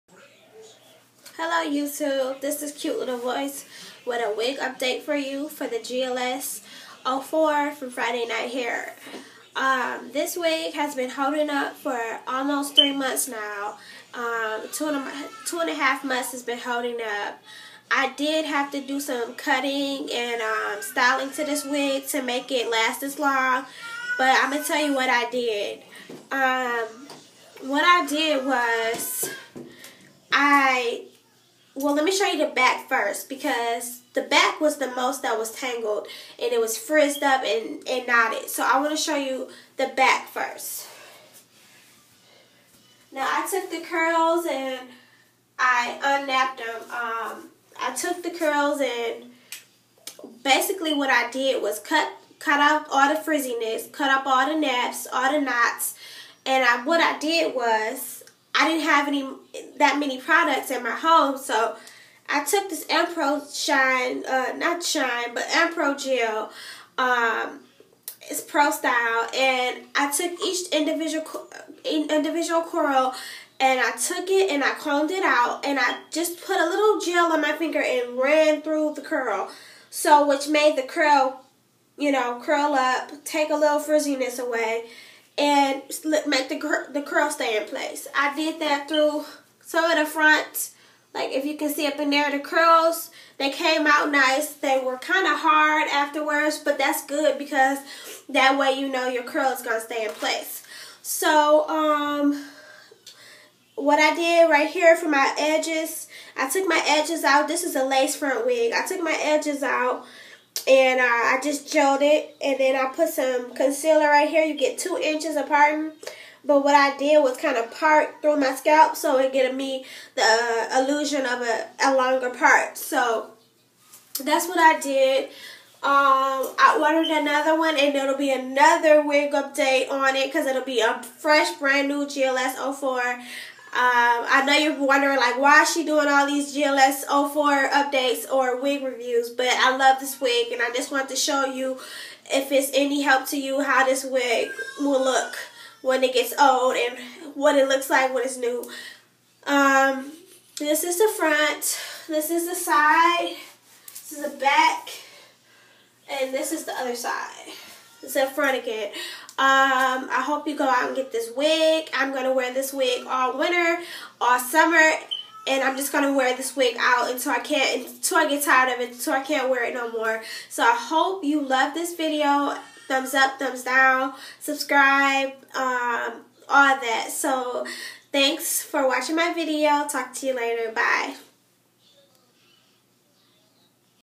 Gls 04 2month update
Gls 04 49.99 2inch parting short curly hairstyle 10-12 inches great hair, great quality for the price